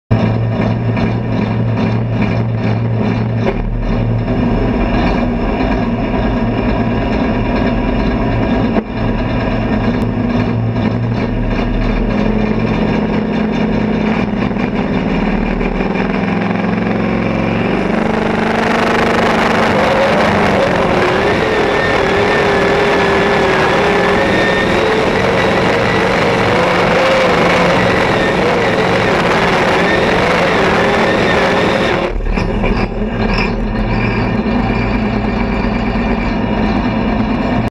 Bucksnort Placing 4th at Redwood 2014
Andrea Muller driving at Redwood Falls.  Placing 4th in the Super Farm class